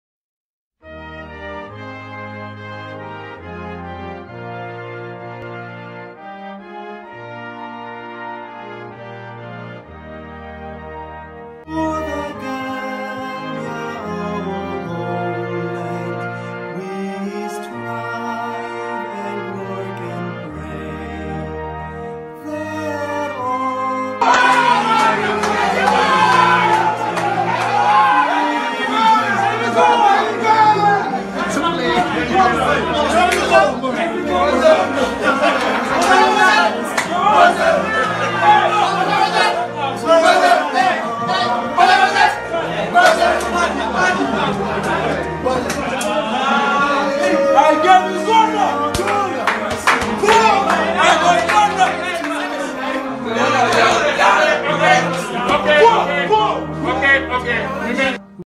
under 20 final the Gambia vs Senegal
congrats to the u-20 for winning the WAFU

waw waw Gambia scorpions